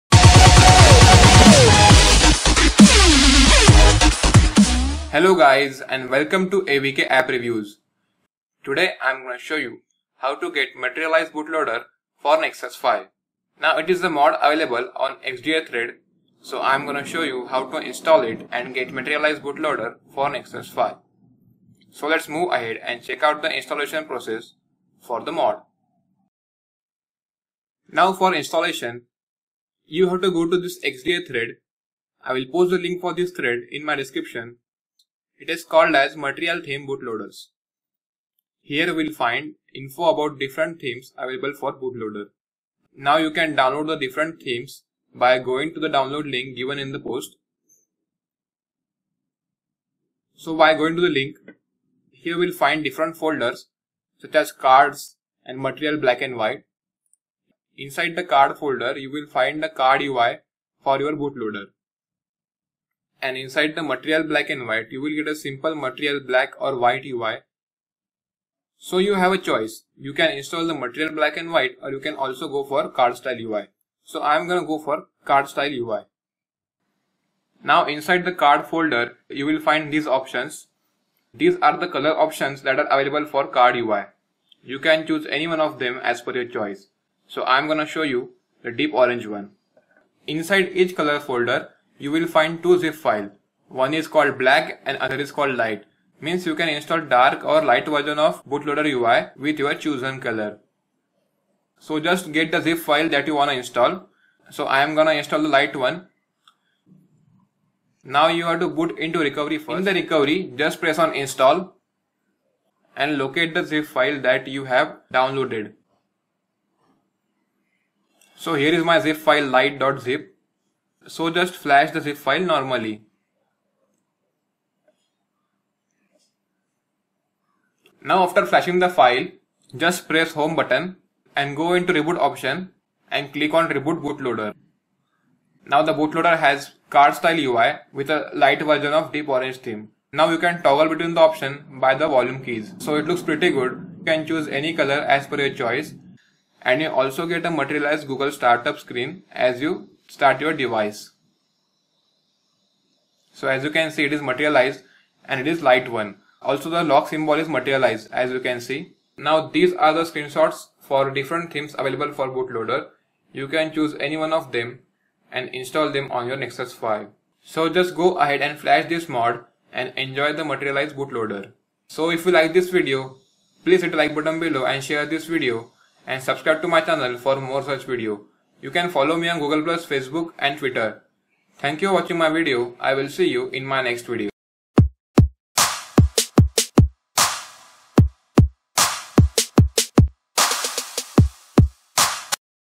Material Bootlaoder for Nexus 5 [How -To] (Noob Friendly)
This is the how-to video about getting material style bootloader for Nexus 5. Its a very simple and easy process. Must try out these Bootloaders.

For restoring back original bootloader , just flash latest bootloader file for Nexus 5 .

Follow me on :
Google+: +AVKAppReviews